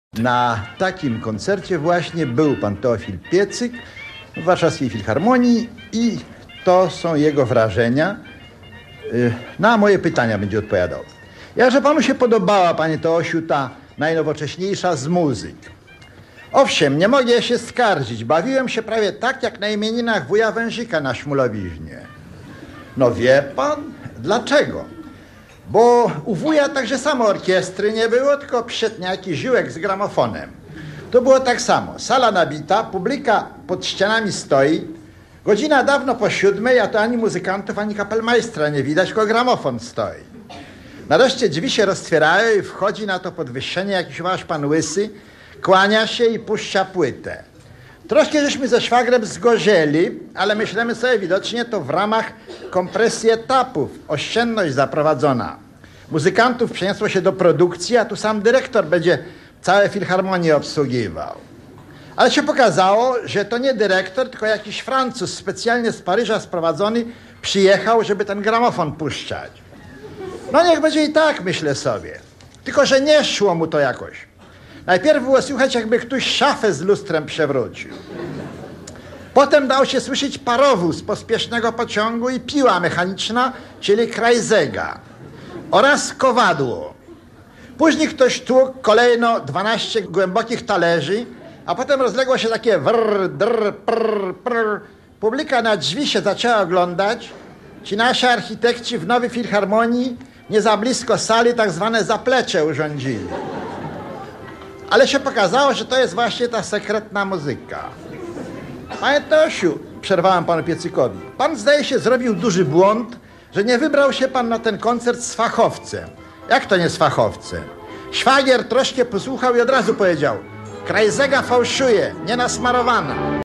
Gwarą warszawską Wiech ustami pana Piecyka recenzuje Warszawską Jesień
Dziś kończy się 62 festiwal Warszawska Jesień. Posłuchajmy co o Warszawskiej Jesieni w roku 1959 mówił bohater Wiecha, pan Teofil Piecyk (ma swoją ulicę w dzielnicy Targówek, a Stefan Wiechecki - ulicę w Śródmieściu i park na Targówku).